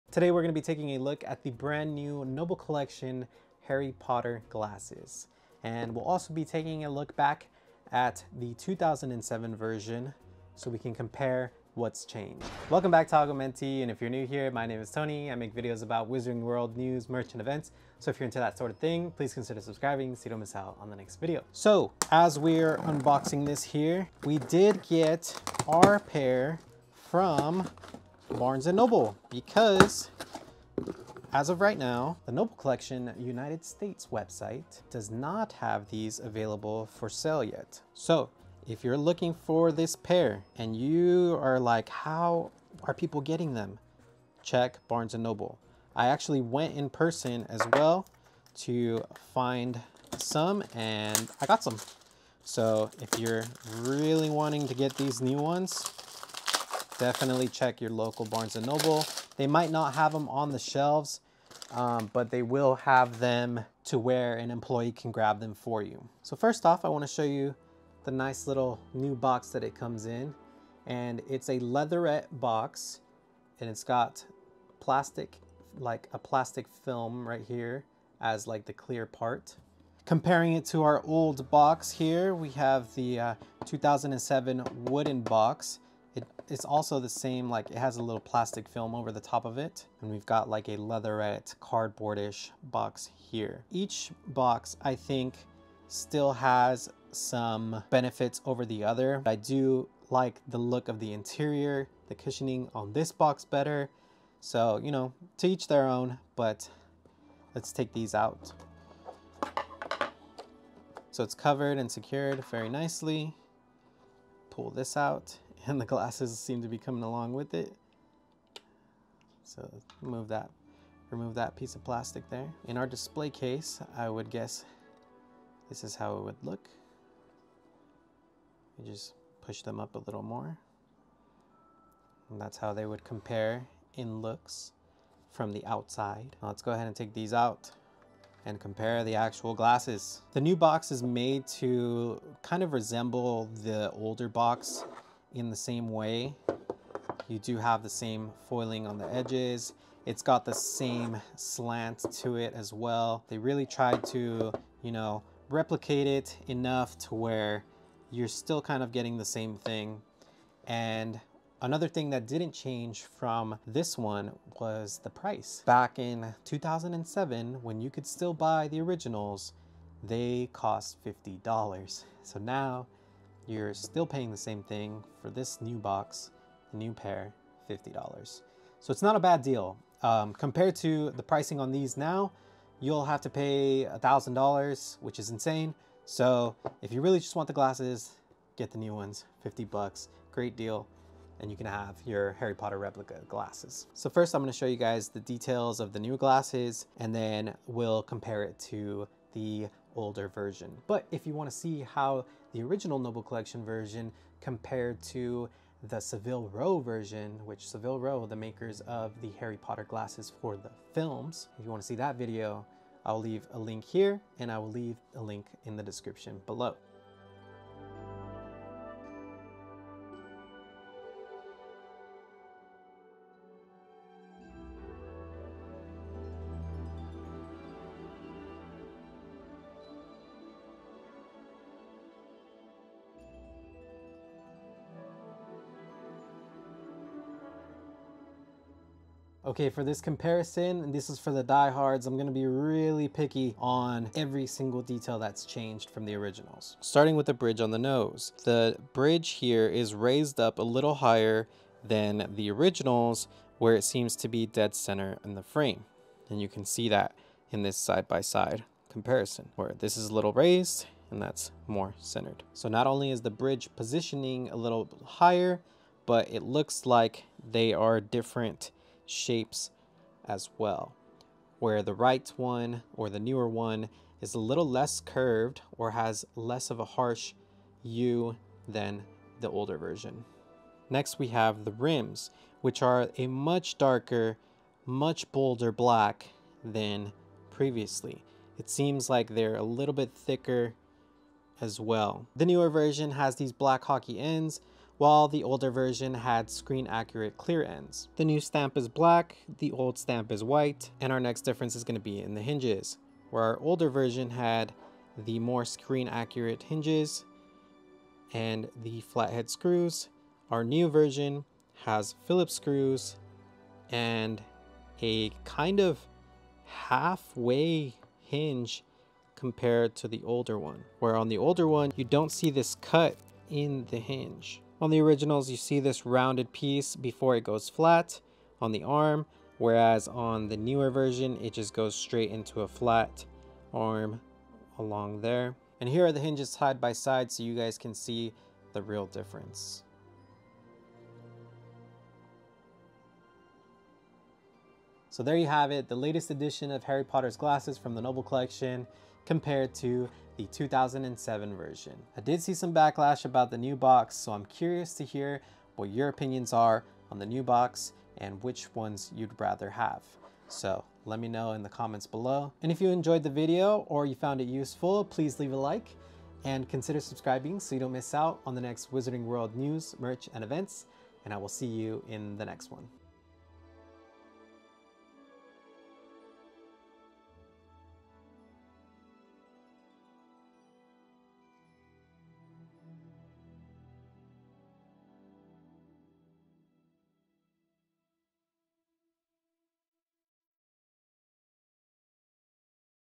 2023 Harry Potter's Glasses Replica Unboxing | The Noble Collection
Today we'll be taking a look at the re-imagined Harry Potter Glasses by The Noble Collection! We'll also be comparing them to their 2007 equivalent. This one is for all the fans who need their items to be as close to screen accurate as possible. I hope this helps!